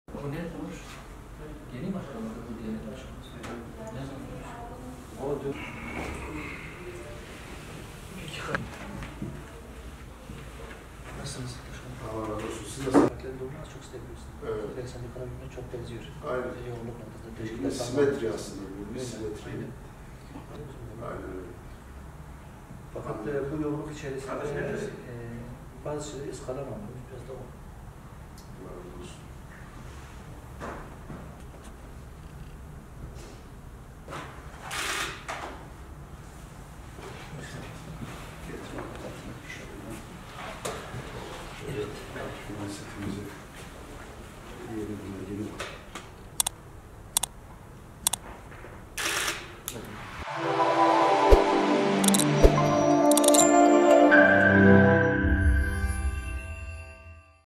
HÜDA PAR'dan Memur-Sen Genel Merkezine ziyaret
​HÜDA PAR Genel Sekreteri Şehzade Demir, beraberindeki heyet ile birlikte Memur-Sen Konfederasyonu Genel Merkezini ziyaret etti.

Değerli İzleyicilerimiz;

TÜRKÇE

KÜRTÇE-KURMANCI

ARAPÇA

İNGİLİZCE

FARSÇA

KÜRTÇE-SORANİ